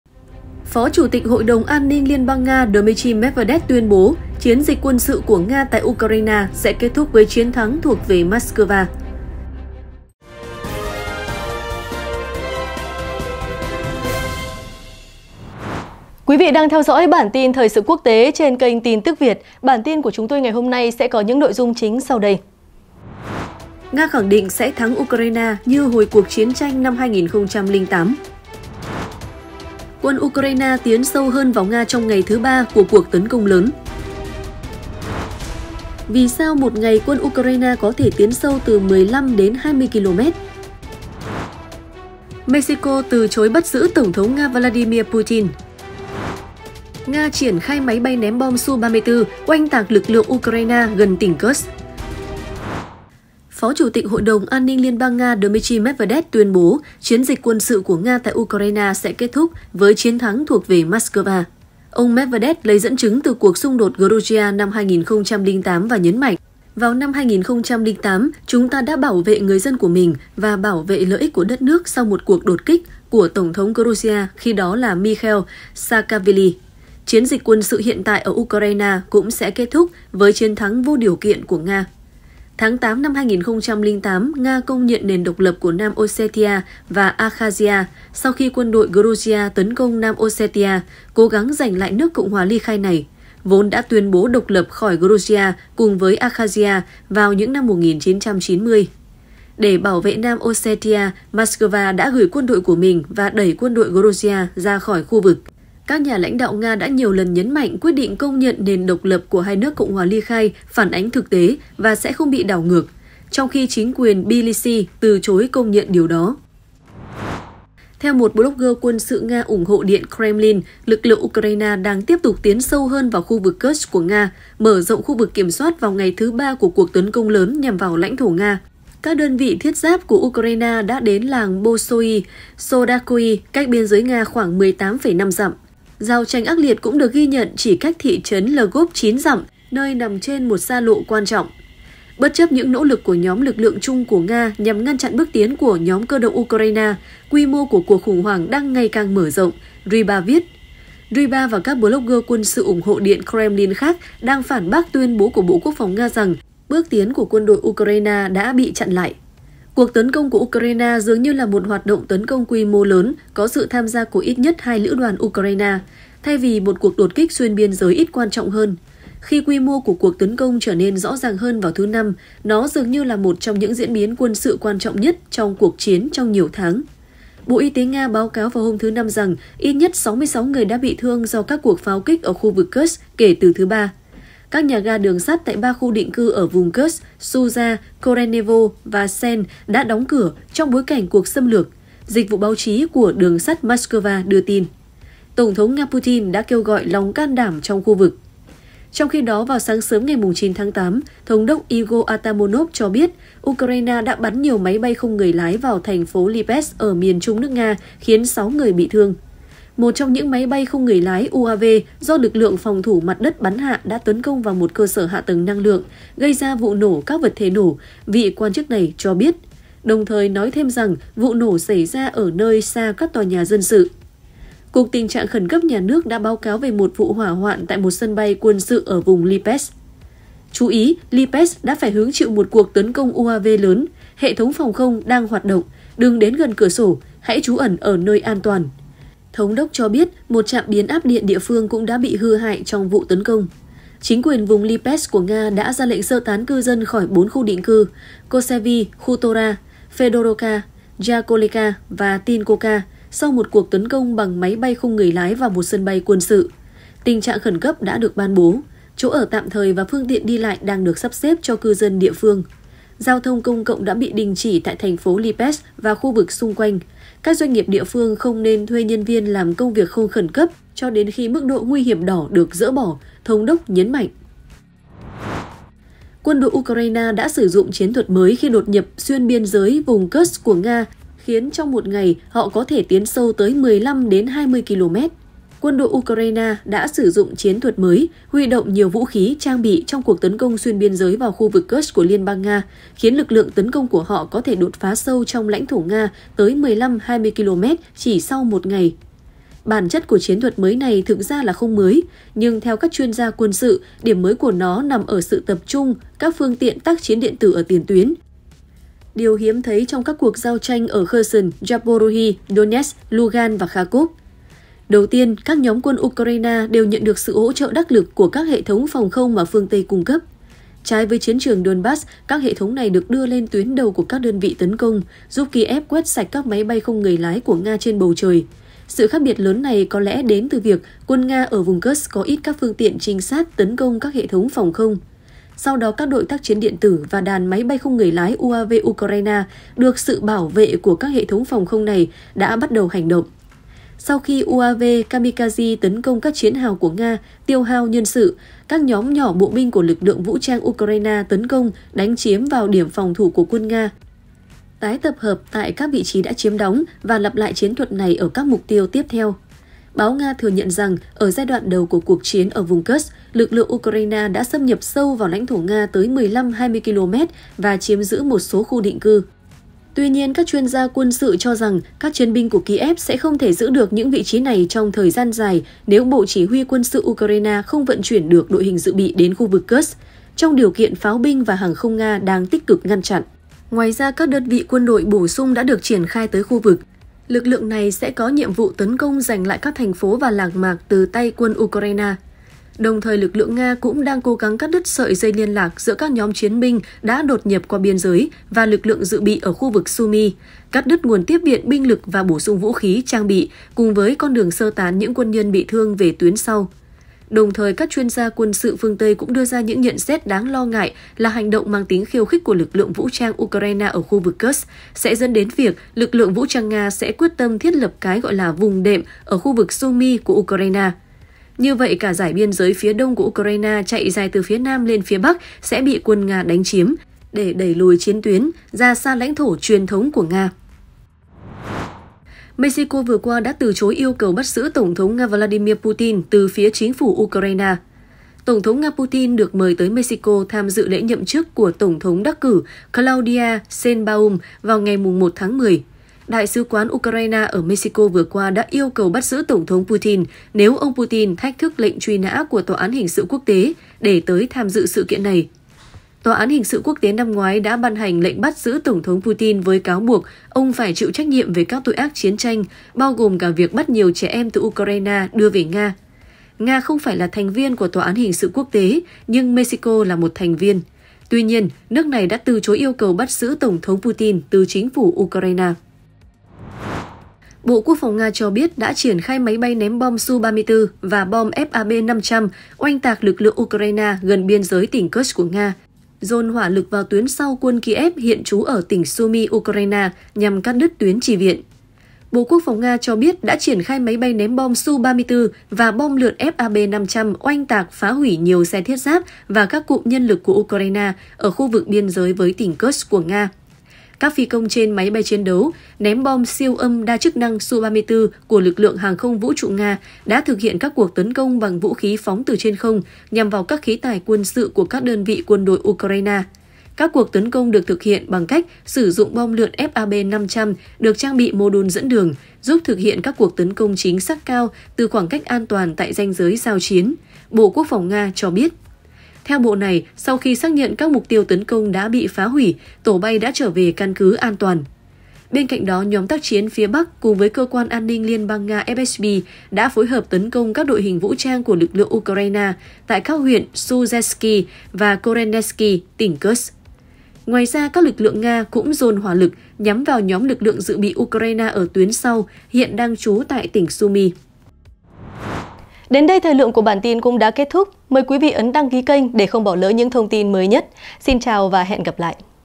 Mexico từ chối "THẲNG MẶT" lệnh bắt giữ Tổng thống Nga Vladimir Putin
Mexico từ chối "THẲNG MẶT" lệnh bắt giữ Tổng thống Nga Vladimir Putin @TinTucVietOfficial

01:01 Nga khẳng định sẽ thắng Ukraine như hồi cuộc chiến tranh năm 2008
02:03 Quân Ukraine tiến sâu hơn vào Nga trong ngày thứ 3 của cuộc tấn công lớn
04:44 Vì sao 1 ngày quân Ukraine có thể tiến sâu 15-20km?
07:18 Mexico từ chối bắt giữ Tổng thống Nga Vladimir Putin
08:04 Nga triển khai máy bay ném bom Su-34 oanh tạc lực lượng Ukraine gần tỉnh Kursk